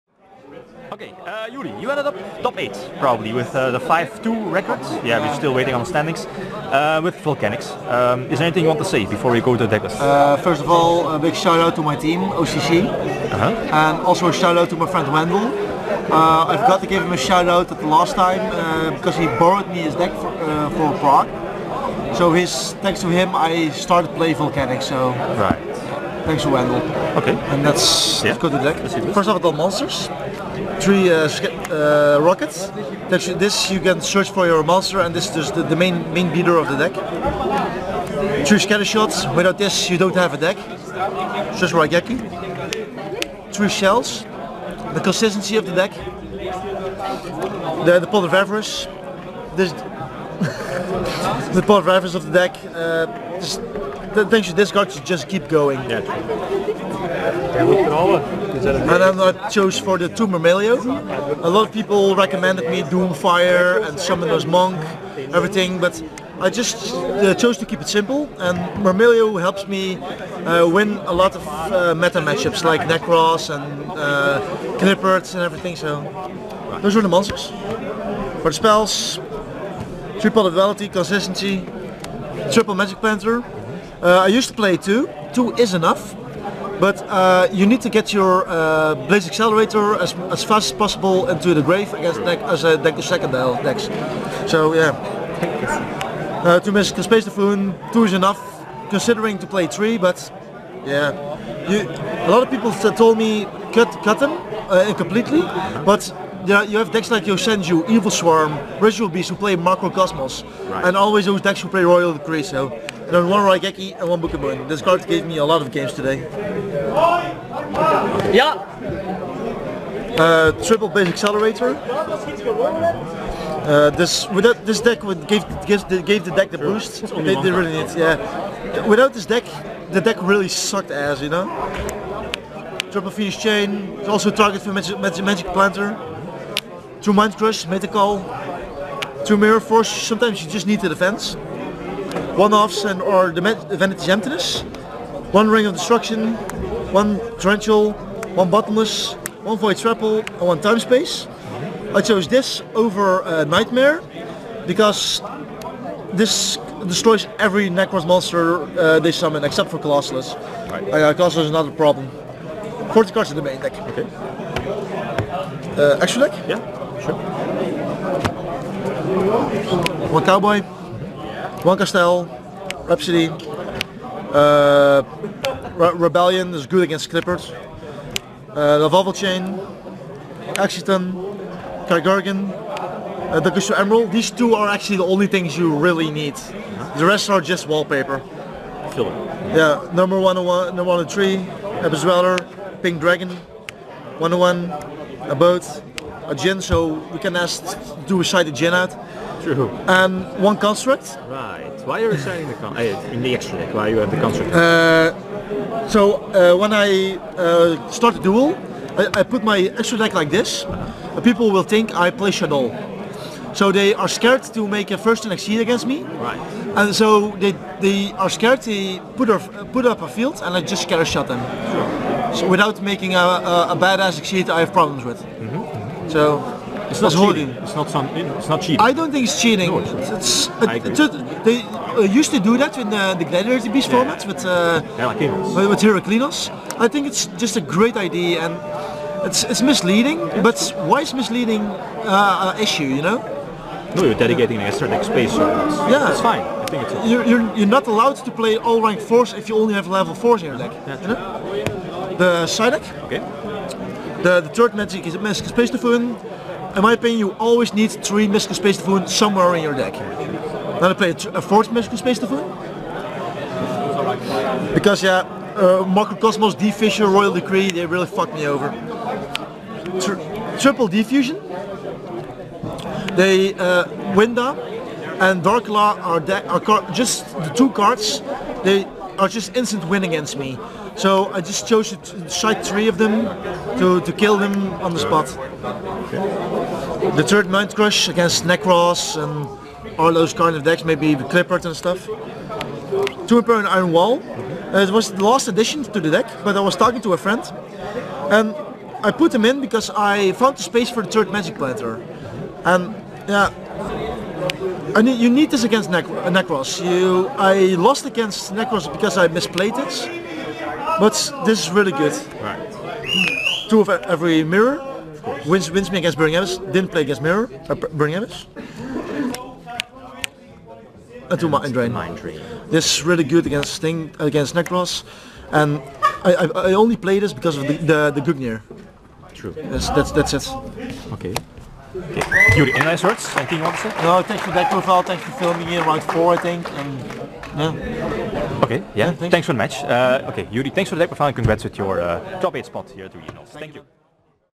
Yu-Gi-Oh! Deck Profile - Regionals (April 2015) - Volcanic Traptrix (5-2)
Maaseik Regionals (April 12 2015) - Volcanics (LIVE) decklist (5-2) by Youri Lansman.. Dat sidedecked Shaddoll Construct though..

Team homepage HERE!